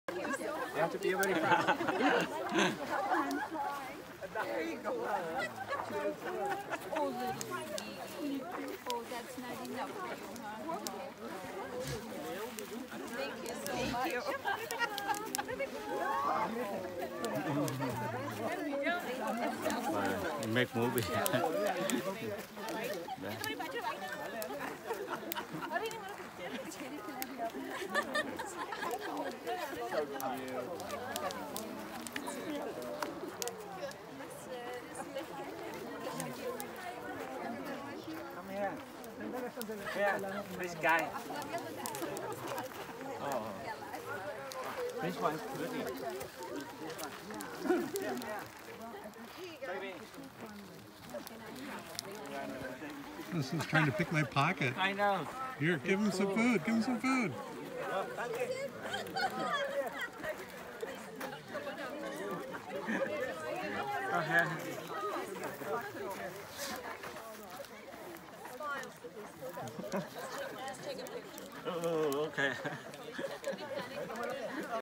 Playing with Reindeer in Finland
IMG 6727. Part of Cosmos Arctic Adventure bus trip